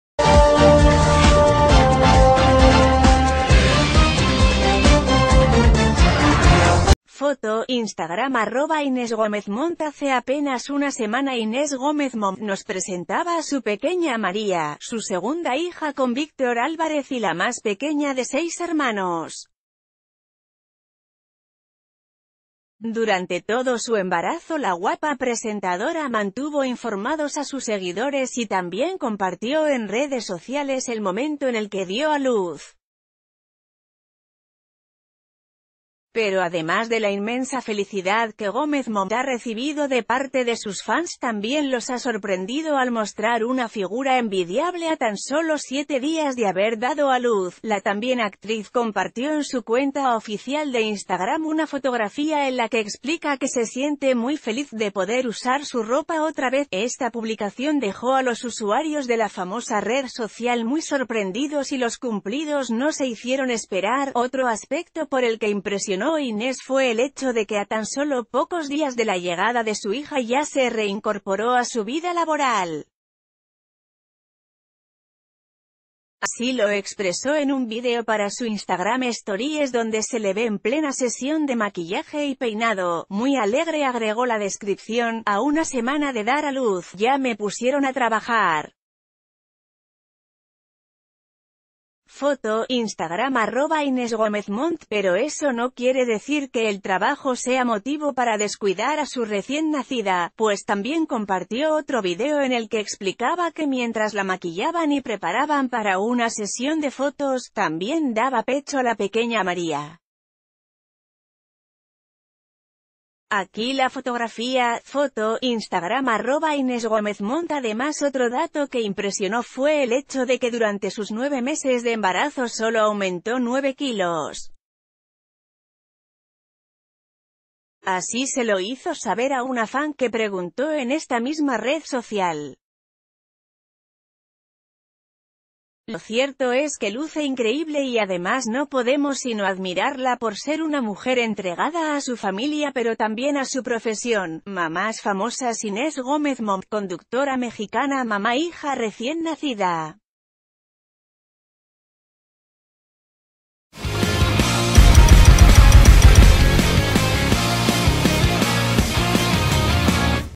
Inés Gómez Mont: a una semana de dar a luz presume cinturita
Foto: Instagram @inesgomezmont Hace a penas una semana Inés Gómez Mont nos presentaba a su pequeña María, su segunda hija con Víctor Álvarez y la más pequeña de seis hermanos.